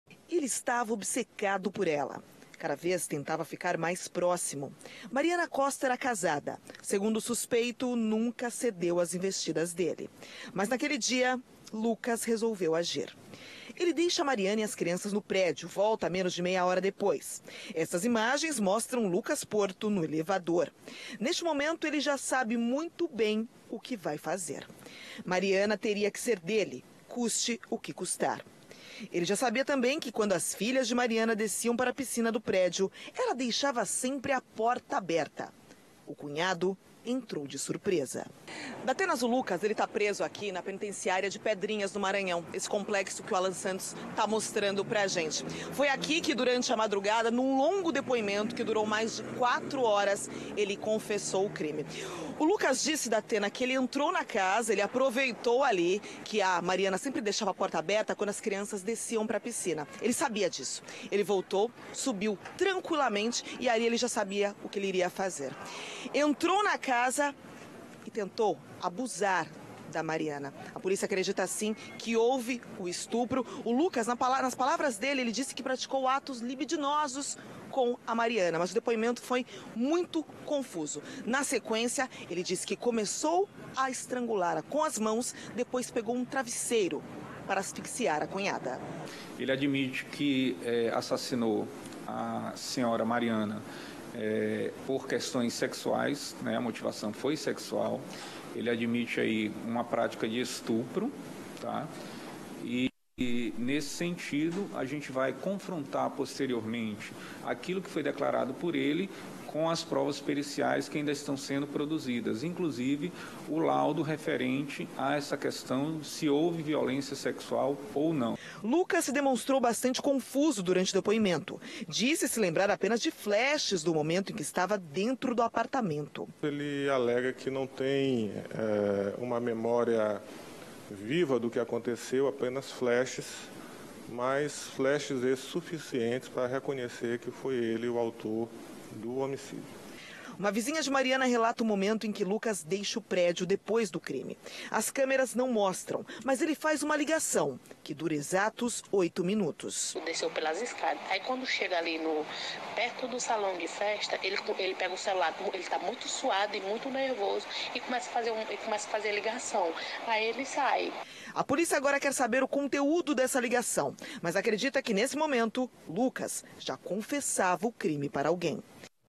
Cunhado de sobrinha-neta de Sarney confessa o crime
O criminoso era obcecado pela vítima. O cunhado da vítima confessou que estrangulou e asfixiou a mulher após tentar abusar dela. Ele está preso na penitenciária de Pedrinhas, no Maranhão.